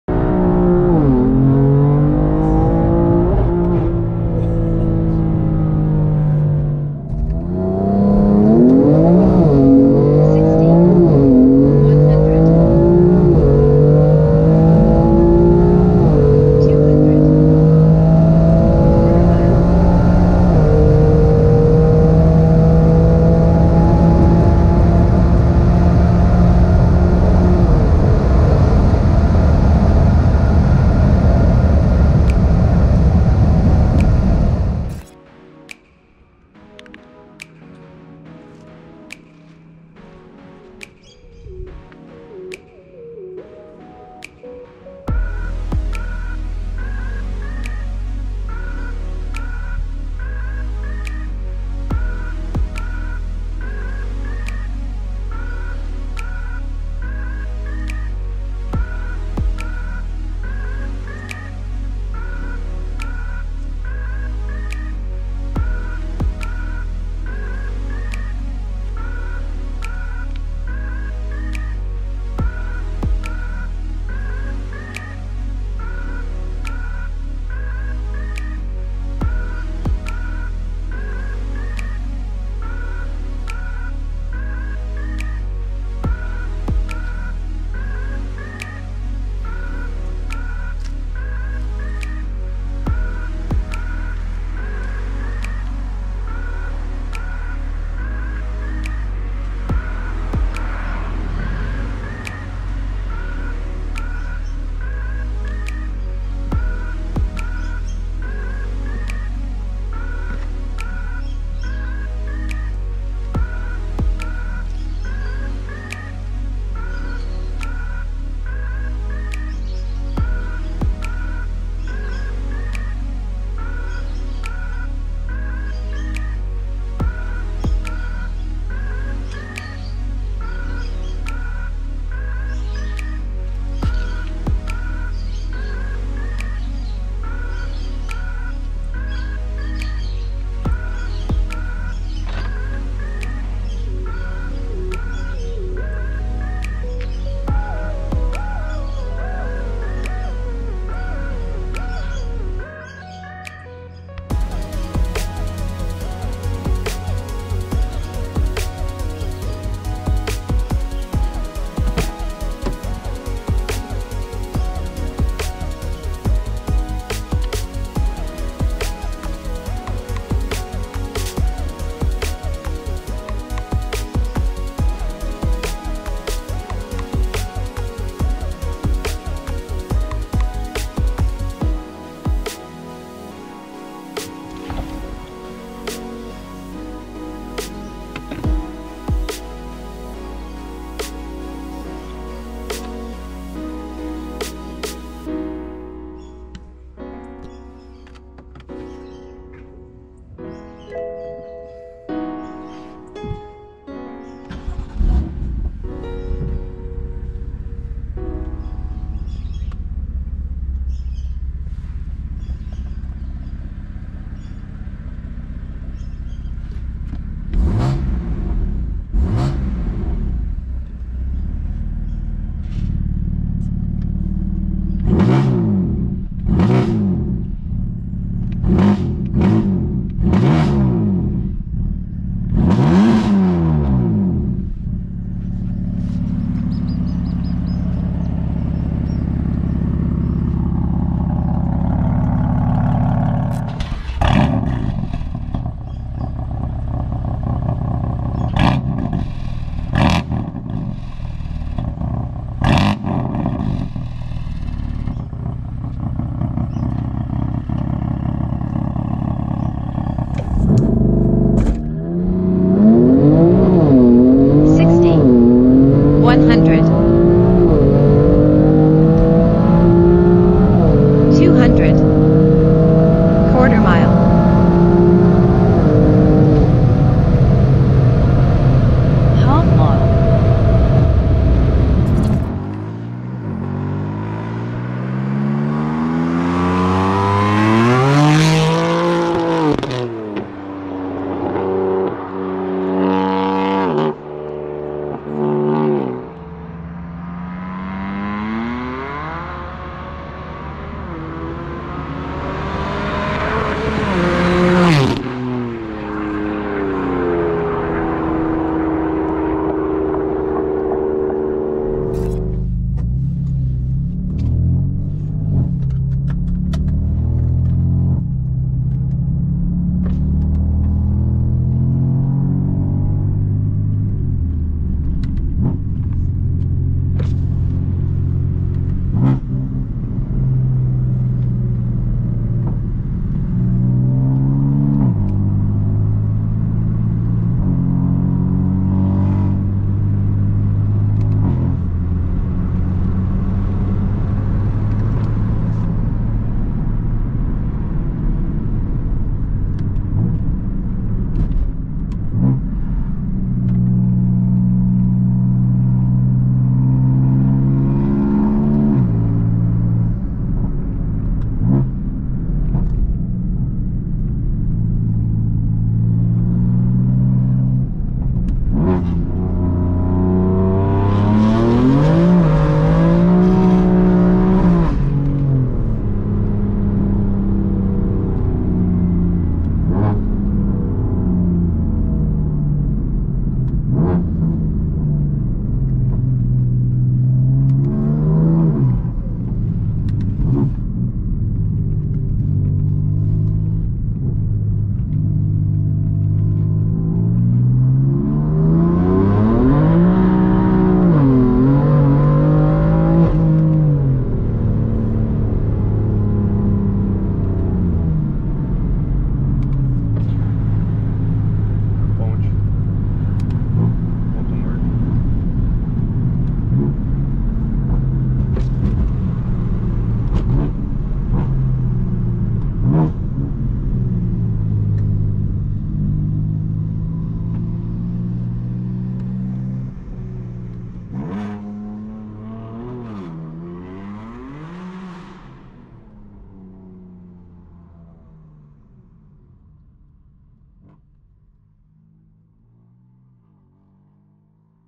BMW M3 700WHP ACELERANDO FORTE!!! 0-311Km | 100-200 DRAGY | ATM 700 AUTOMOTIVE MOTORSPORTS
BMW M3 2015 S55 BY AUTOMOTIVE MOTORSPORTS BRASIL | ATM700

- KIT PURE TURBOS STG2
- CRANK HUB
- EMBREAGEM DODSON
- WATERCOOLER CSF
- REMAP DE CAMBIO
- WMI AEM
- INTAKE INJEN
- PIPES